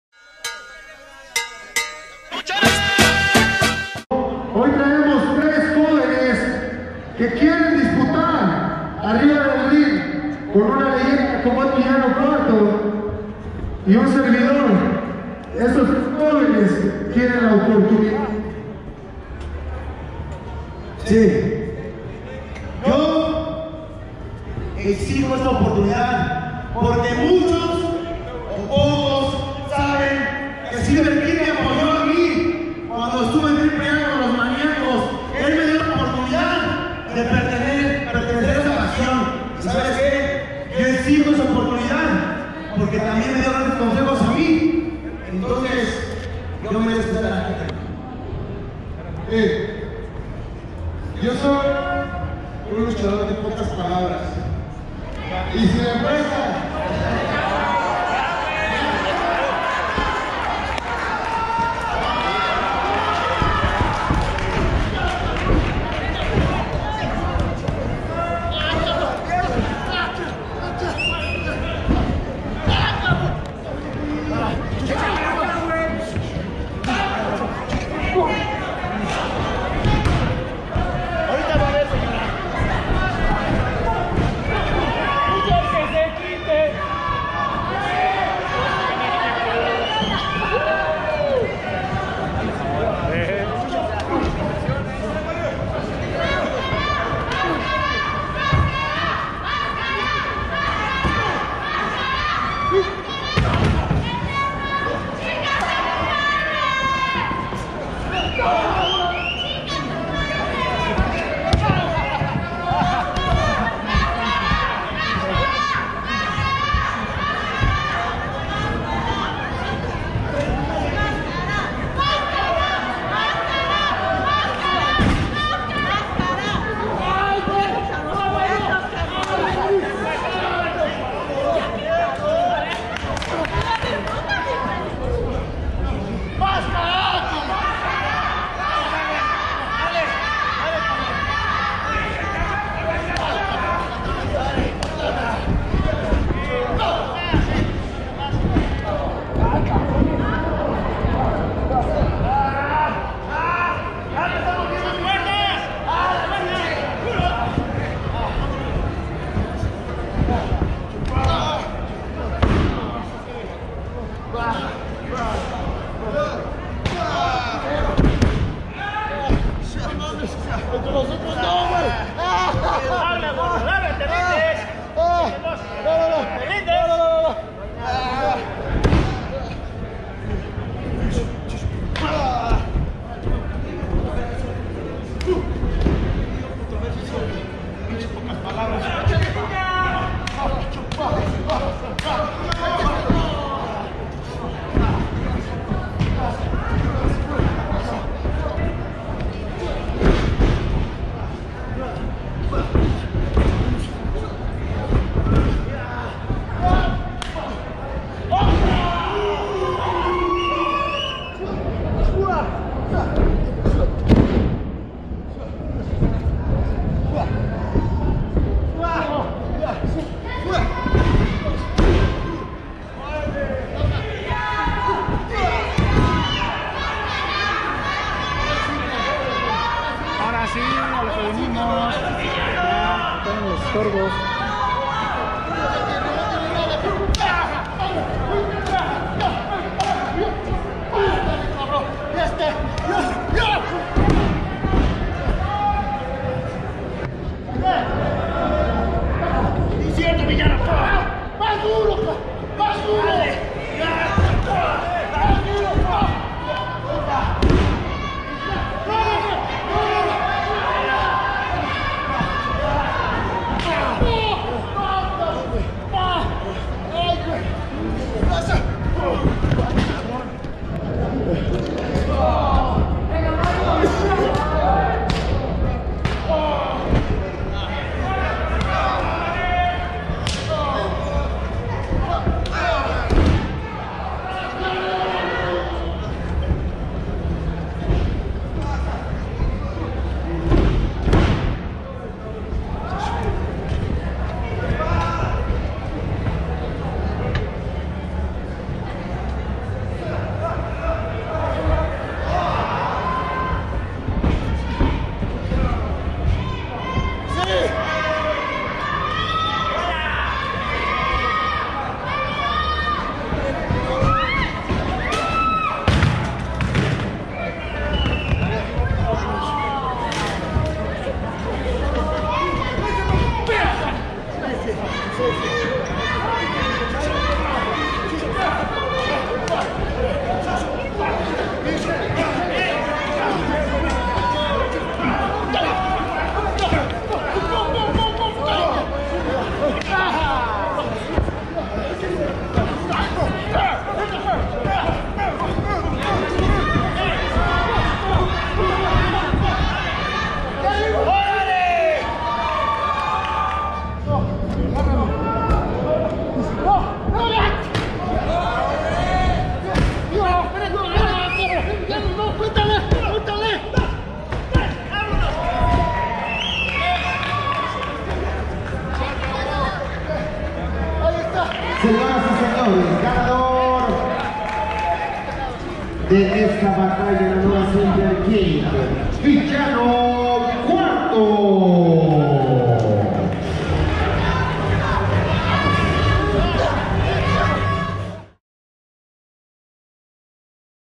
Villanos IV Vs Máscara Año 2000 Jr Vs Eterno Vs H. Sangre Chicana Vs Warrior Jr
El ídolo de la dinastía imperial se lleva la victoria en un duelo que se convirtió en una oportunidad para Eterno, Hijo de Sangre Chicana y Warrior Jr, quienes no estaban originalmente en su lucha ante Mascara Año 2000 Jr...